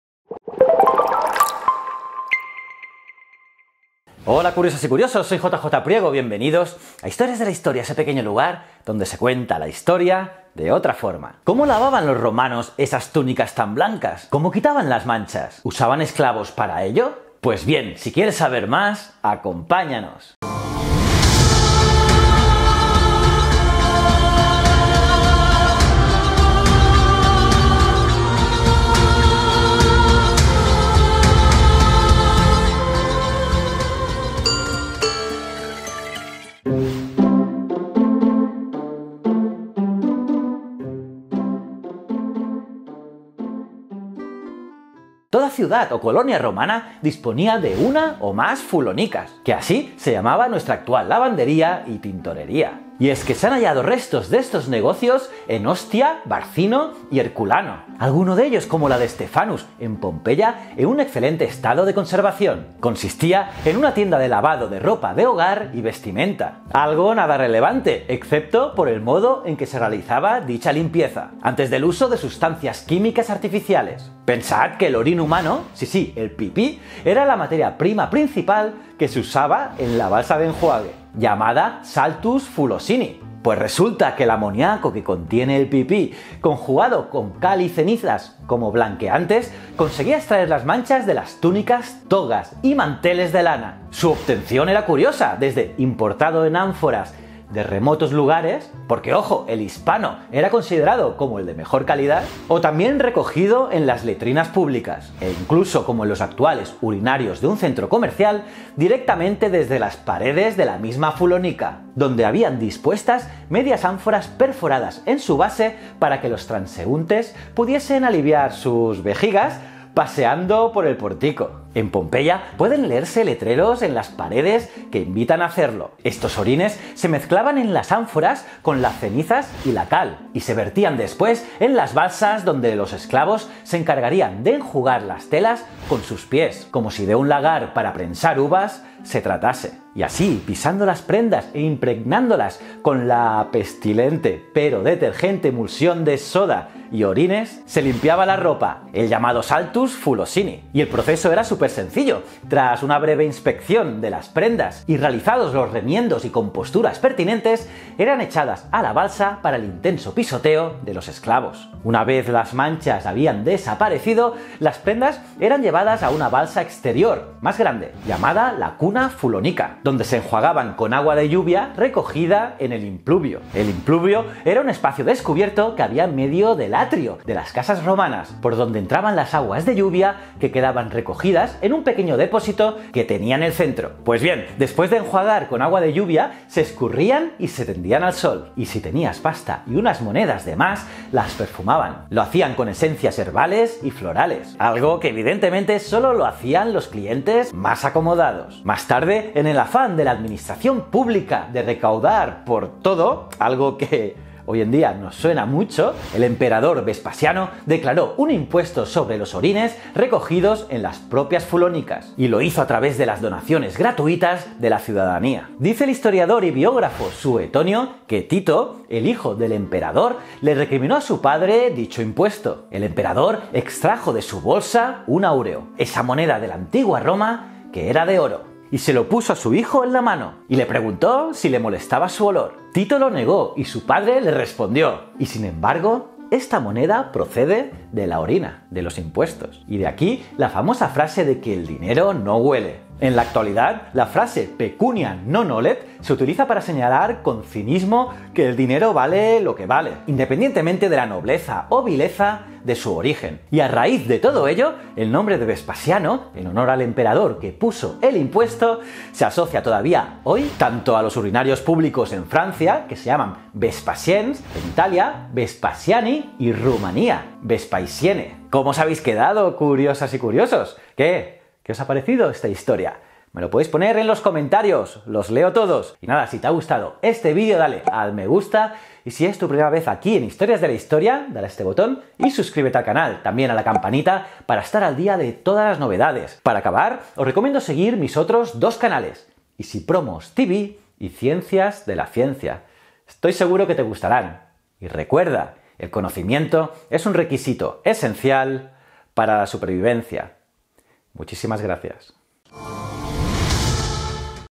👕¿Cómo LAVABAN LA ROPA en la ANTIGUA ROMA? 🤔
¿Cómo lavaban, los romanos, sus túnicas  tan blancas? ¿Cómo quitaban  las manchas? ¿Usaban esclavos  para ello?

𝓜𝓾𝓬𝓱𝓪𝓼 𝓰𝓻𝓪𝓬𝓲𝓪𝓼

𝓙𝓙 😘👌